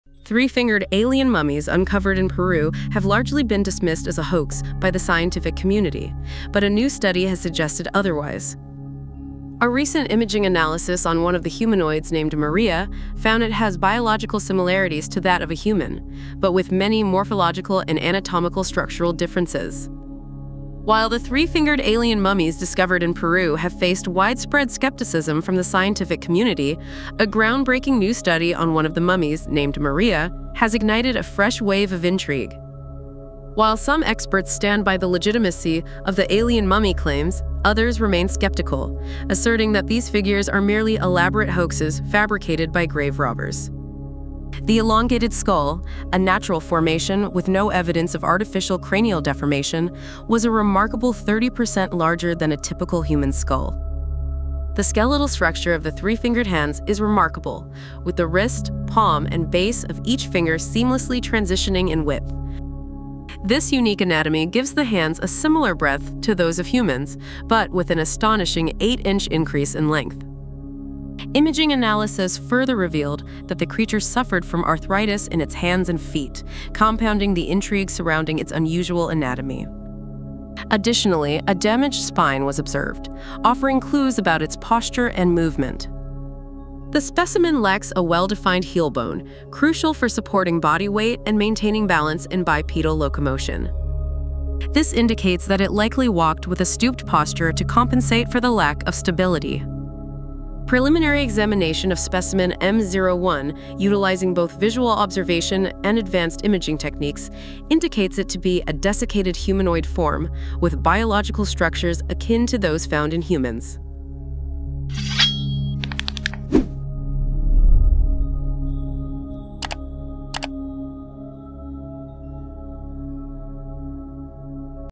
🧬 Decoding Maria: The 'Alien' Mummy's DNA & Anatomical Puzzles
Is this the proof we've been waiting for? 👽 A new study suggests the "alien" mummy "Maria," discovered in Peru, might actually have human-like biological structures with significant anatomical differences. This shocking revelation challenges everything we thought we knew about ancient history and human evolution.

In this video, we dive deep into the mysteries surrounding Maria:

🔬 Human-Like Biology: Uncover the scientific evidence suggesting Maria's connection to humanity.

💀 Anatomical Differences: Explore the 3-fingered hands, elongated skull, and other unique features that set Maria apart.

🕰️ Nazca Connection: Could Maria's existence be linked to the enigmatic Nazca civilization?

🤔 Alien or Human Evolution? What does this discovery mean for our understanding of human origins?

Join us as we unravel the secrets of Maria and explore the possibility that we are not alone in the universe. Don't miss this groundbreaking video!